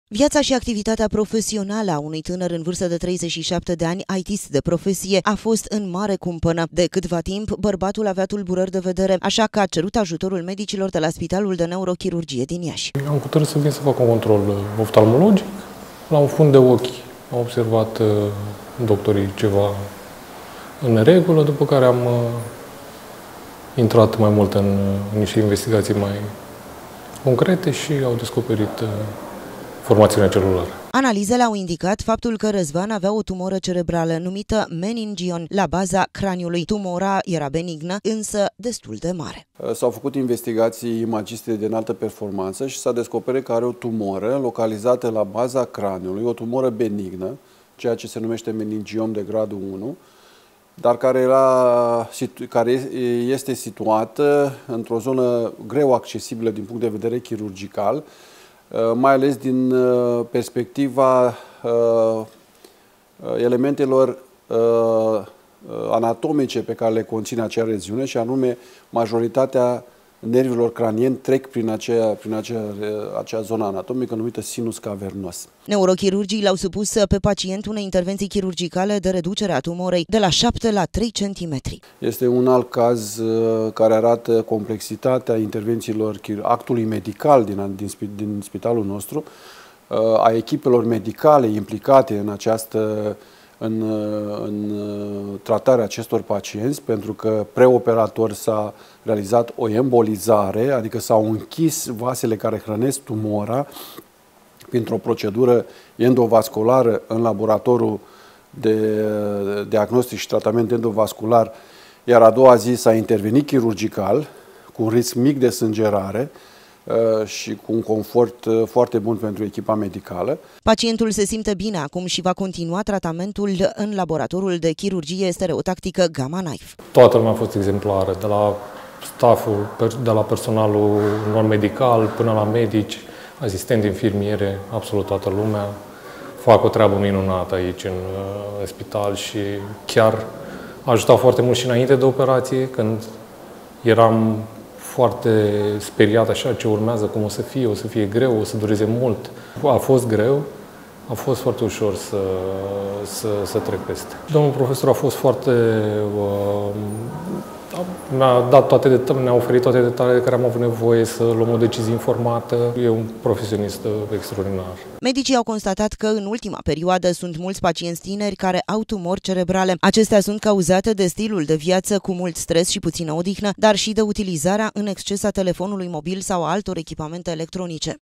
IT-IST CU TUMOARE PE CREIER, SALVAT DE NEUROCHIRURGI
Viata lui Razvan, un IT-ist in varsta de 37 de ani, care suferea de o tumoare la baza craniului, a fost salvata de catre neurochirurgii ieseni. Acestia l-au supus unei interventii chirurgicale complicate, in urma careia au extras cea mai mare parte a tumorii. Razvan va urma si un tratament cu Gamma Knife, absolut necesar in asemenea situatii.